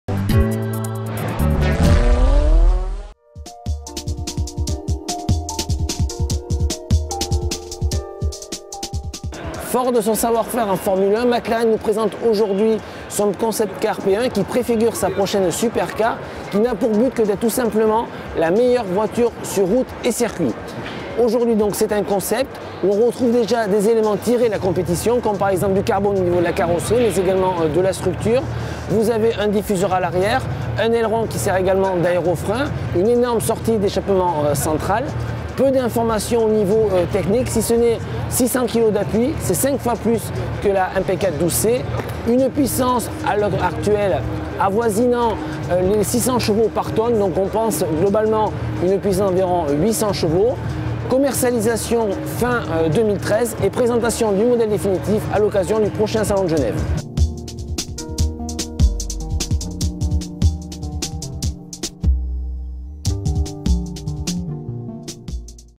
La McLaren P1 en direct du Mondial
Profitant de son expérience en Formule 1, McLaren nous présente aujourd'hui son concept P1 qui préfigure sa future supercar et succéderait ainsi à la mythique F1.